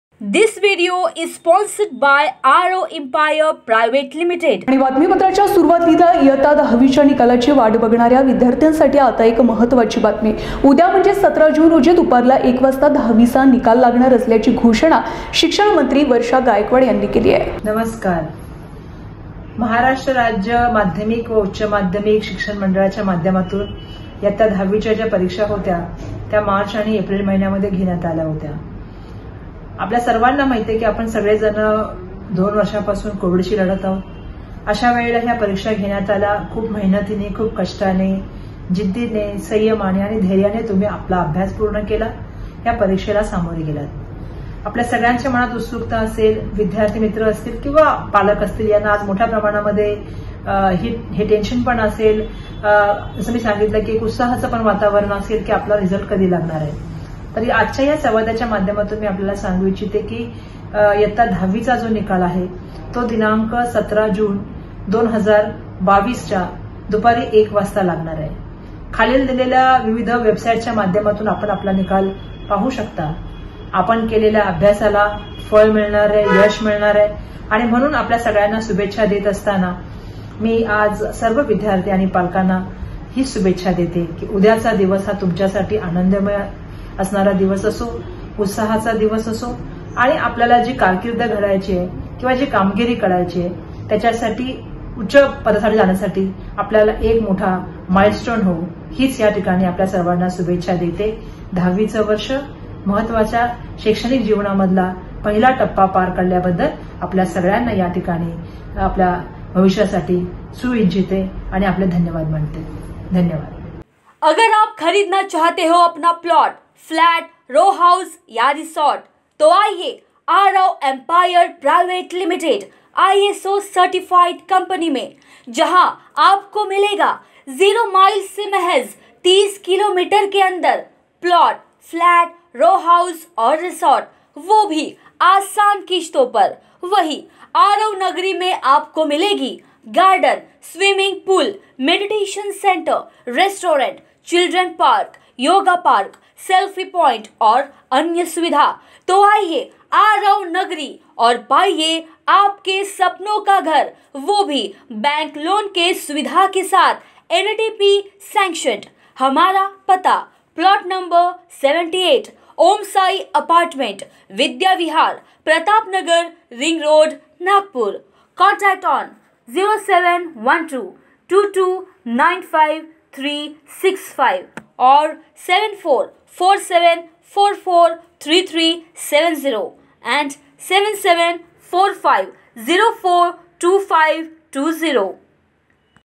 उद्या दुपारी १ वाजता दहावीचा निकाल होणार जाहिर - वर्षा गायकवाड
इयता  दहावीच्या निकालाची वाट बघनार्या विद्यार्थ्यांच्सथी  महत्वाची बातमी .. उदय म्हणजेच १७ जून रोजी दुपारी १ वाजता दहविचा निकाल लागणार असल्याची घोषणा शिक्षान्मंत्री वर्षा गायकवाड यानी केलि आहे...